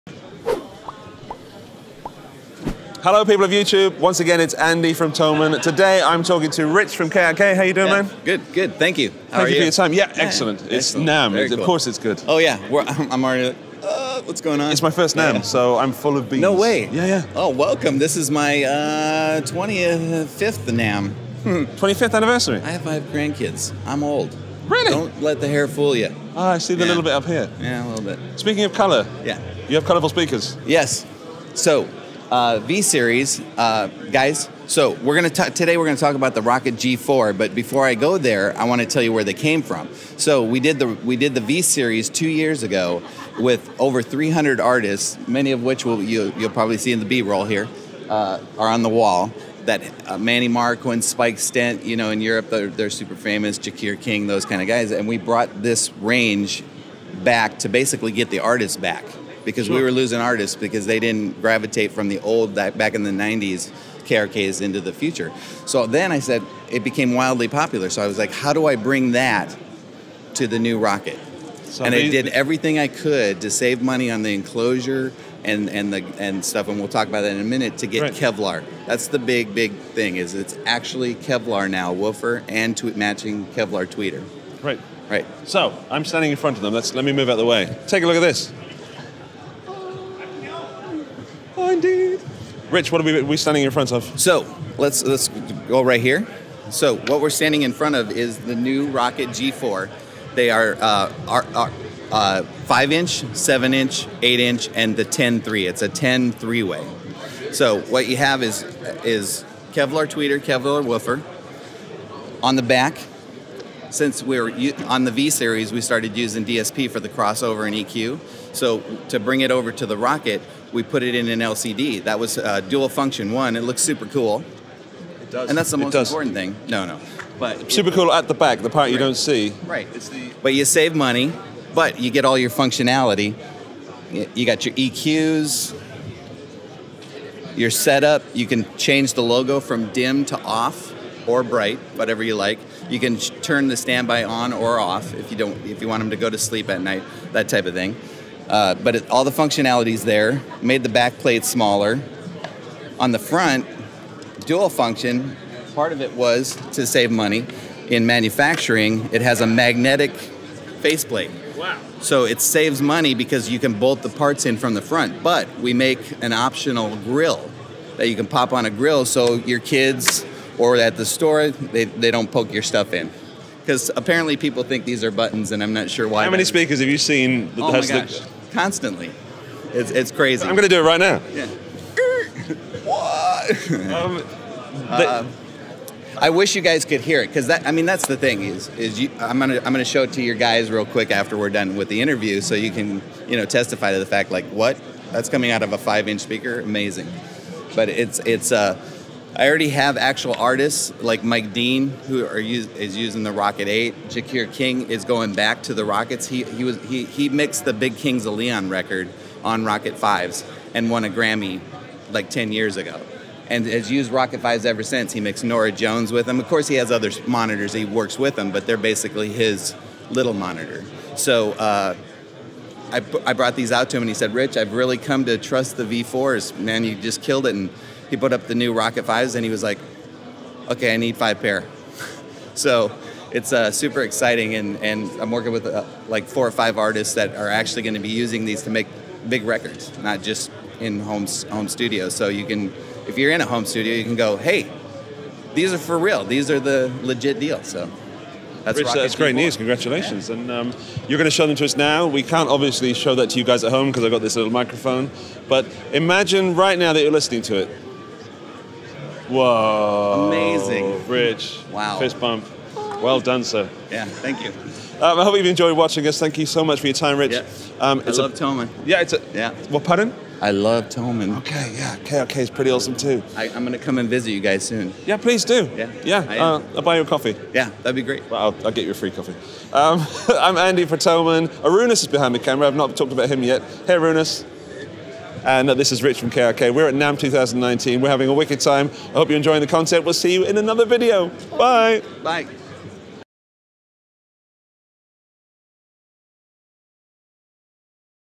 NAMM 2019 | New KRK Rokit Generation 4 Studio Monitors | Thomann
New Completely Rdesigned KRK Rokit Generation 4 Studio Monitors at NAMM 2019.